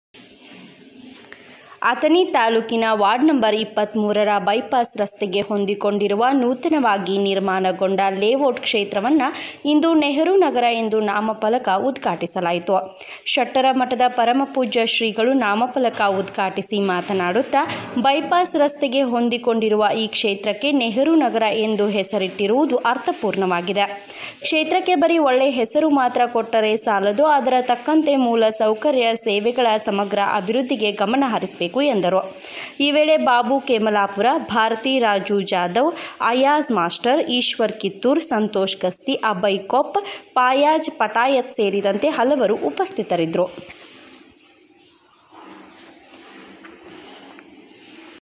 ವಾರ್ಡ ನಂ.23ರಲ್ಲಿ ಶೆಟ್ಟರ ಮಠದ ಪ. ಪು. ಶ್ರೀಗಳಿಂದ ನೆಹರು ನಗರ ನಾಮಫಲಕ ಉದ್ಘಾಟನೆ.
New India TV Kannad #